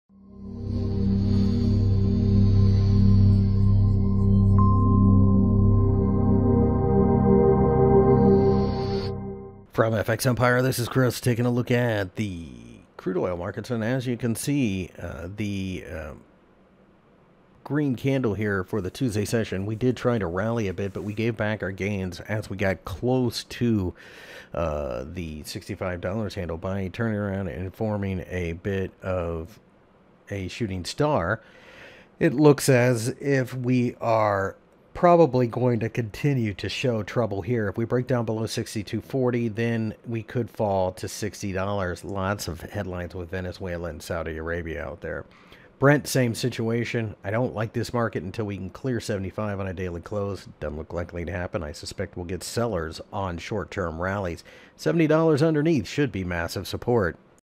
Oil Technical Analysis for May 01, 2019 by FXEmpire.com
Crude oil markets trying to rally during the day on Tuesday but have struggled a bit here and there. There are a lot of... For the full article go - Crude Oil Forecast May 01, 2019, Technical Analysis.